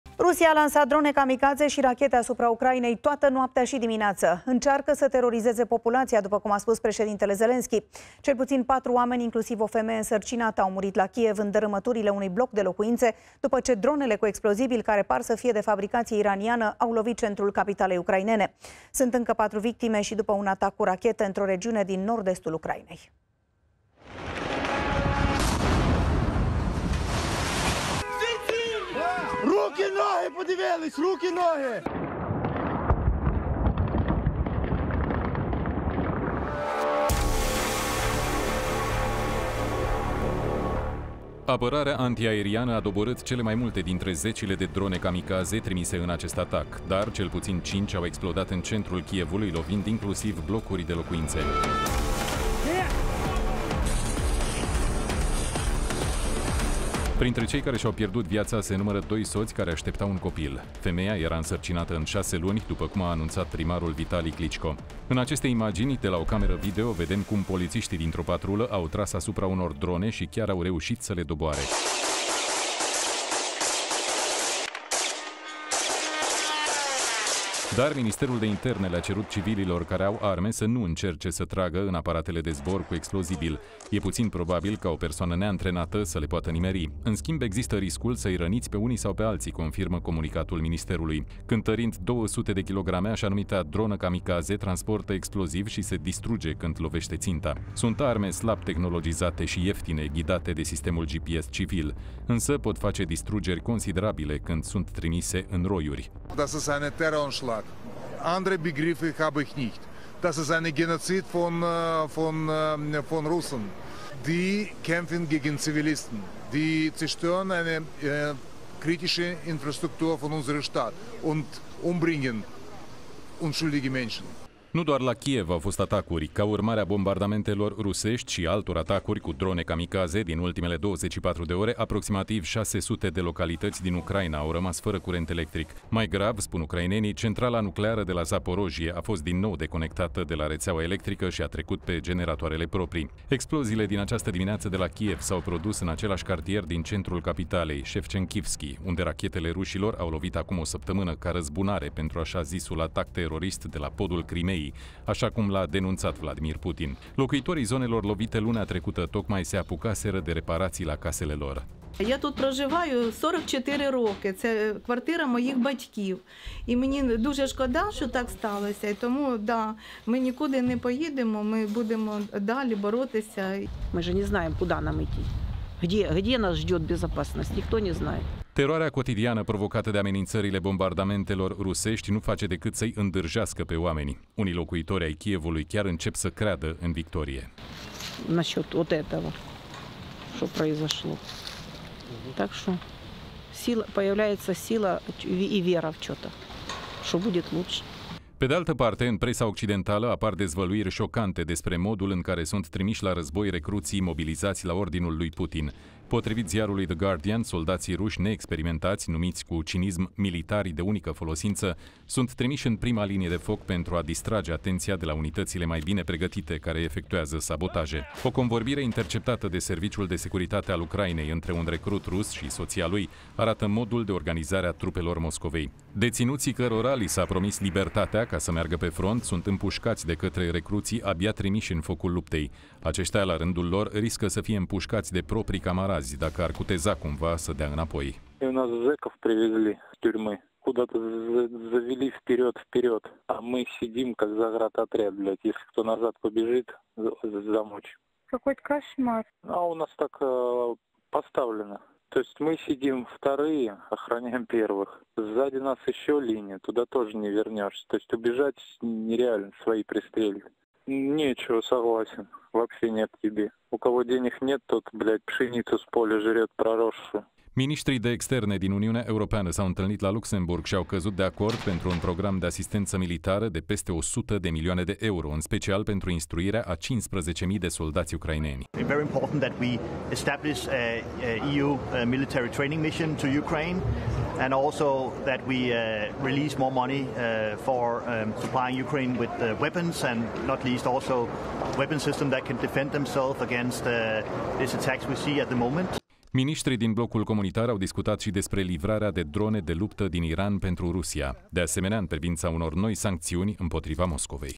Centrala nucleară Zaporojie a fost deconectată din nou de la alimentarea externă cu energie
Rusia a lansat drone kamikaze și rachete asupra Ucrainei în noaptea de duminică spre luni, dar și de dimineață. "Încearcă să terorizeze populația", după cum a spus președintele Zelenski.